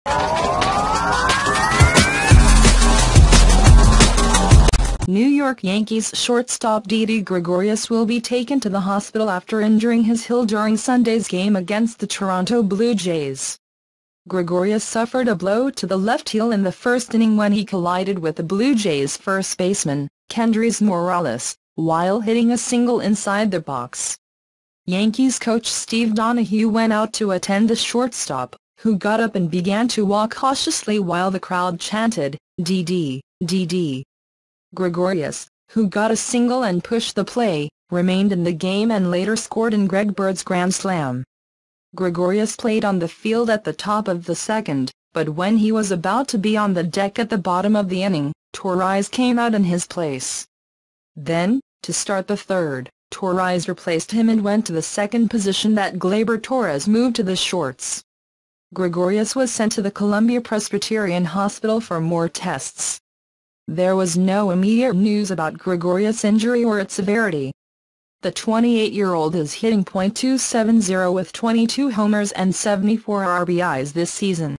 Short field Didi Gregorius of the Yankees will be taken to the hospital after injuring his heel.
New York Yankees shortstop Didi Gregorius will be taken to the hospital after injuring his heel during Sunday's game against the Toronto Blue Jays. Gregorius suffered a blow to the left heel in the first inning when he collided with the Blue Jays' first baseman, Kendrys Morales, while hitting a single inside the box. Yankees coach Steve Donohue went out to attend the shortstop, who got up and began to walk cautiously while the crowd chanted, "Di-Di, Di-Di." Gregorius, who got a single and pushed the play, remained in the game and later scored in Greg Bird's Grand Slam.

Gregorius played on the field at the top of the second, but when he was about to be on the deck at the bottom of the inning, Torreyes came out in his place. Then, to start the third, Torreyes replaced him and went to the second position that Gleyber Torres moved to the shorts. Gregorius was sent to the Columbia-Presbyterian hospital for more tests. There was no immediate news about Gregorius' injury or its severity. The 28-year-old is hitting .270 with 22 homers and 74 RBIs this season.